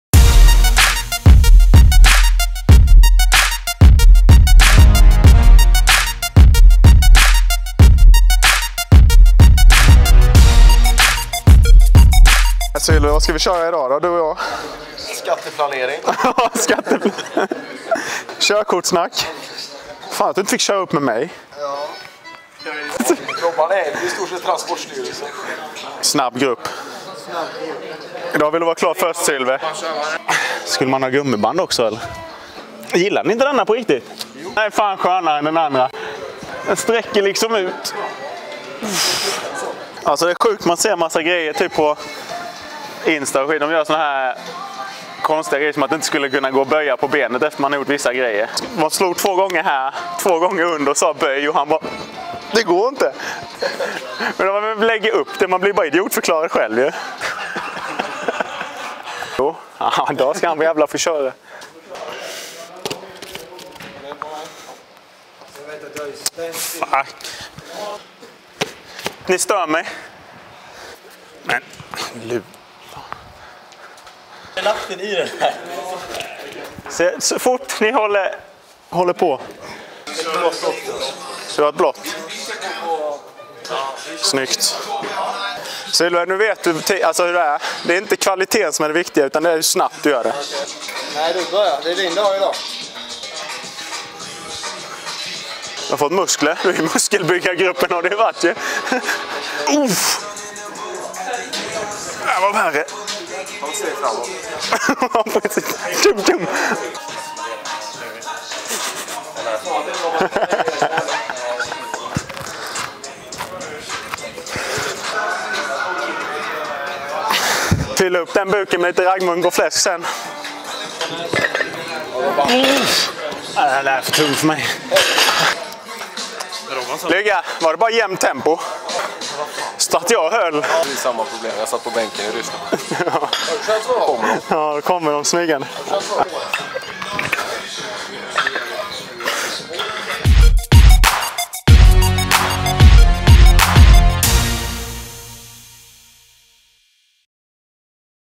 En dag med Robert Rosén
Förra veckan kunde han presenteras för kommande tre säsonger i Växjö Lakers. Häng med trotjänaren under ett träningspass i en dag med Robert Rosén.